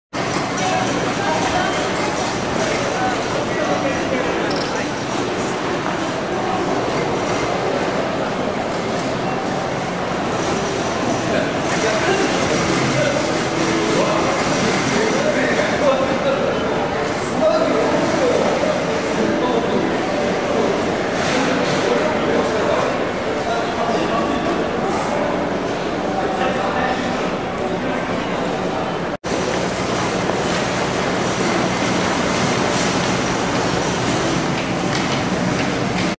仙台駅 グランクラス展示とはやぶさデビューカウントダウン
2011年2月撮影。
グランクラス展示は仙台駅や新青森駅などに置かれていましたね。
当時はスーパーグリーンという仮称だったのが懐かしいです。